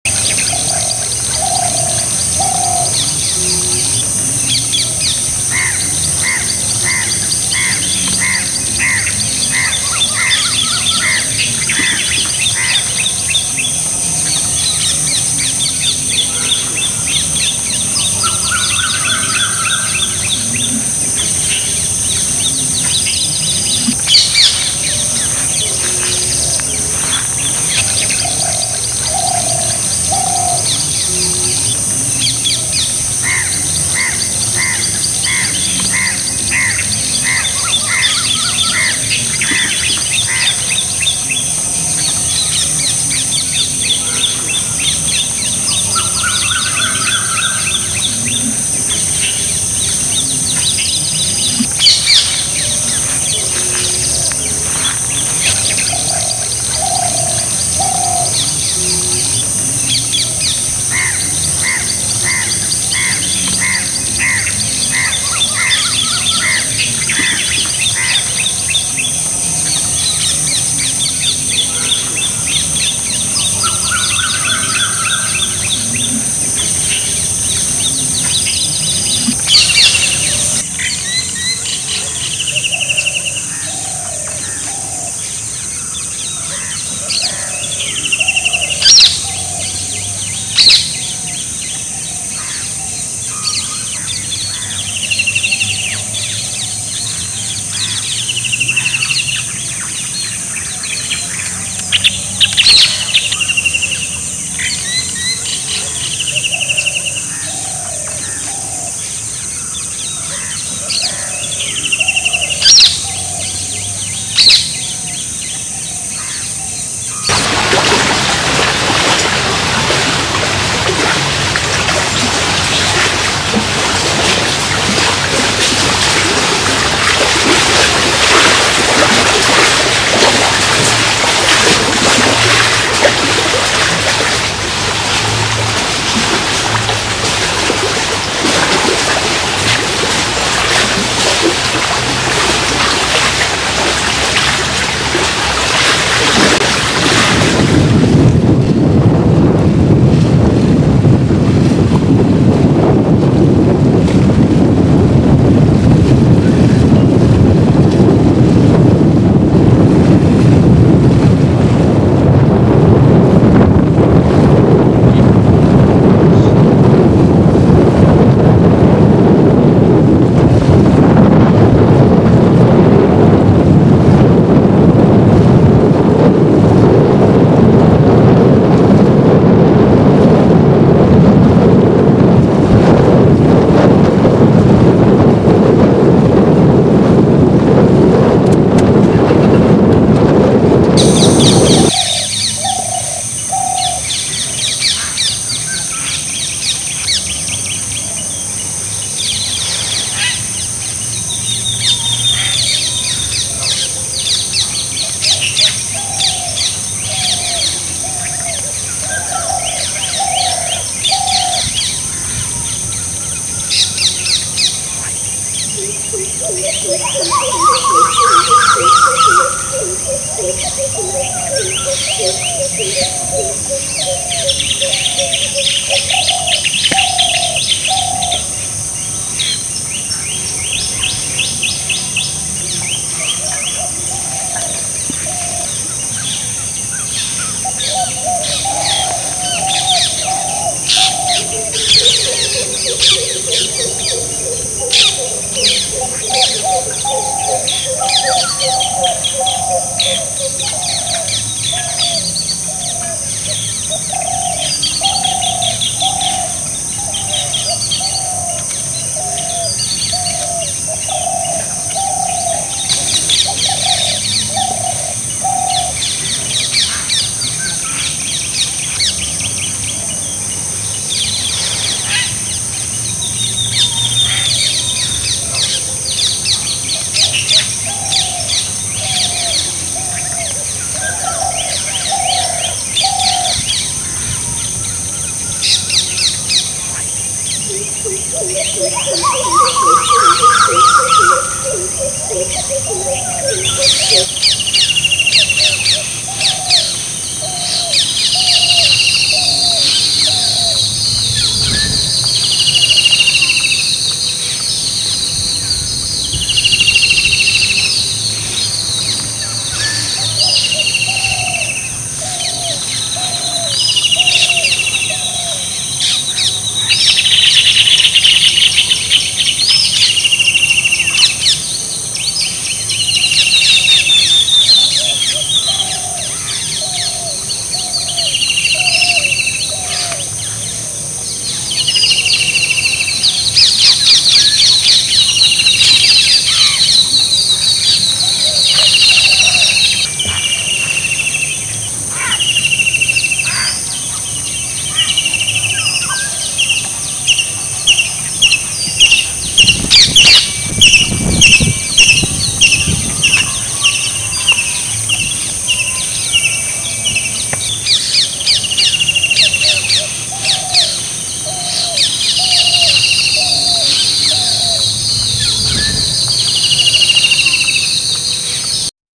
Asian Openbill , Black winged Stilt ,Pond Heron ,Black headed Ibis ,Grey Heron & Egrets
I was able to film these beautiful water birds in their natural habitats during my holidays..

Staring - Asian Openbill , Black winged Stilt ,Pond Heron ,Black headed Ibis ,Grey Heron and Egrets...